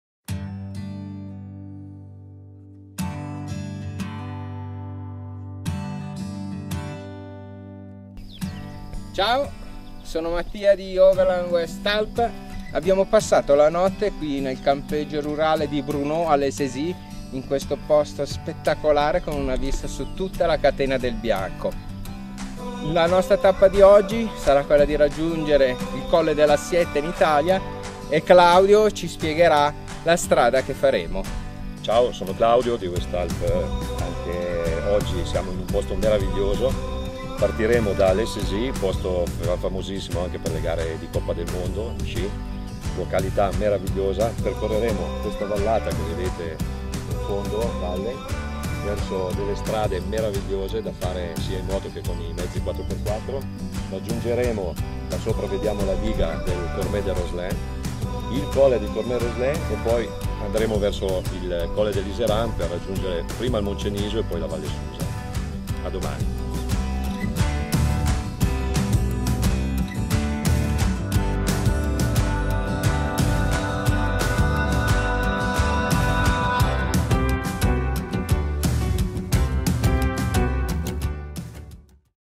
11 GIUGNO - Col de Saisies, P.so e lago del Moncenisi, Colle delle Finestre, Strada dell'Assietta
Un viaggio avventura dal Monte Bianco al mar ligure percorrendo strade panoramiche, sterrate e militari alla ricerca di luoghi e panorami nascosti alla ricerca della bellezza di questi luoghi con un pizzico d'avventura e tanta passione per il fuoristrada.